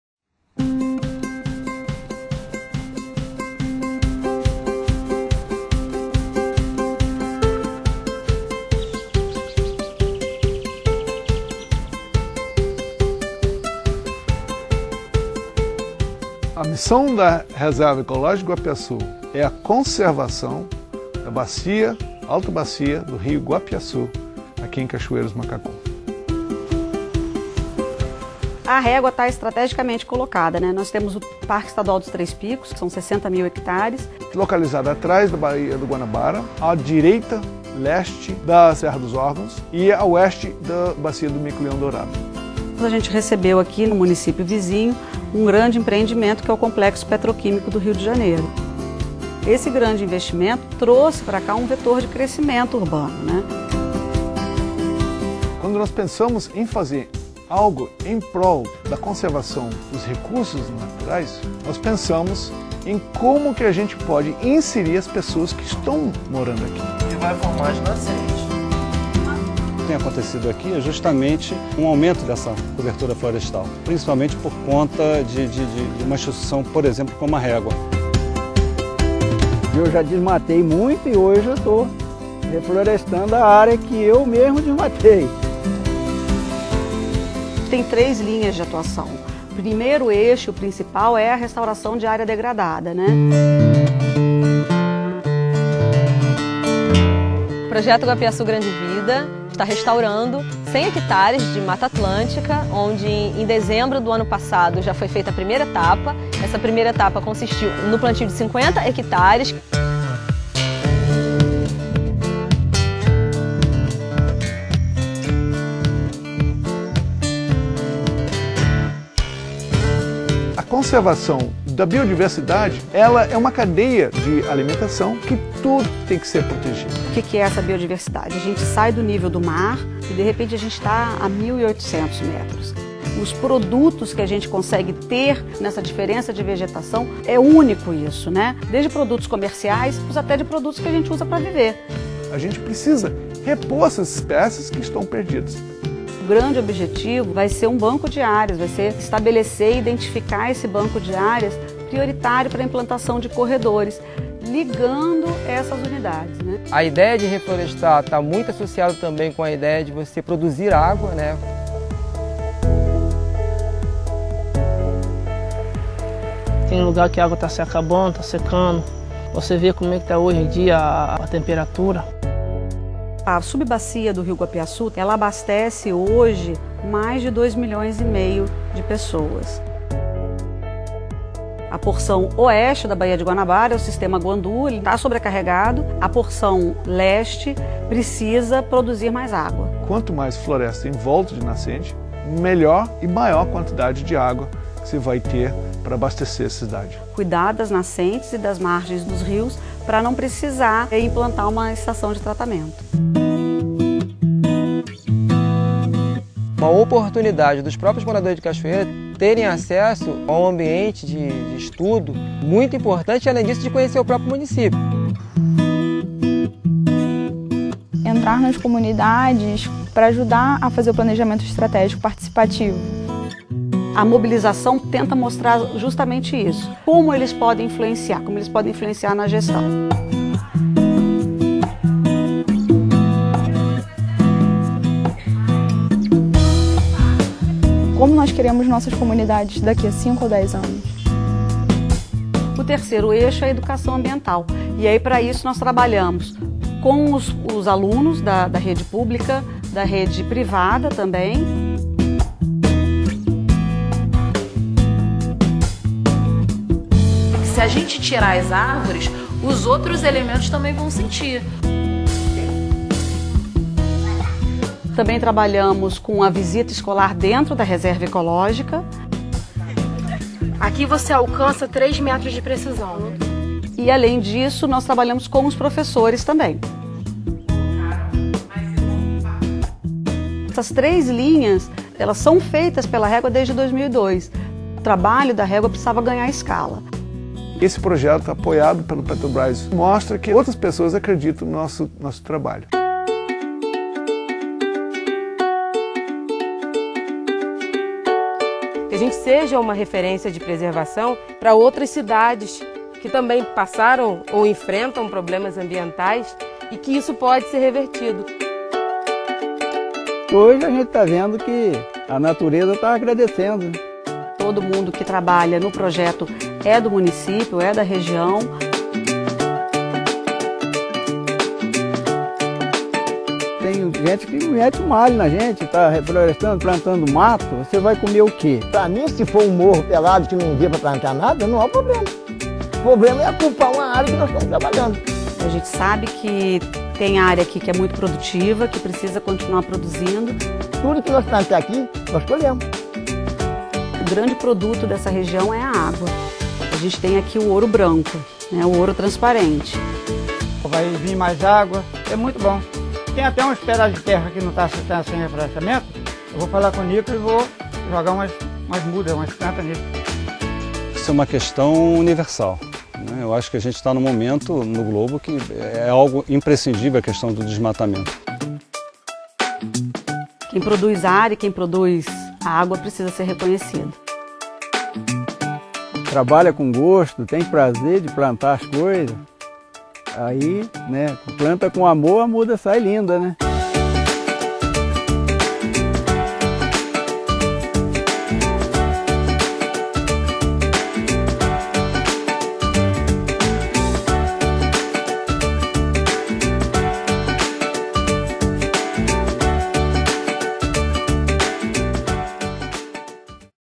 Projeto Guapiaçu Grande Vida
Vídeo institucional do projeto Guapiaçu Grande Vida, realizado pela REGUA com o patrocínio da Petrobras, através do Programa Petrobras sócio ambiental.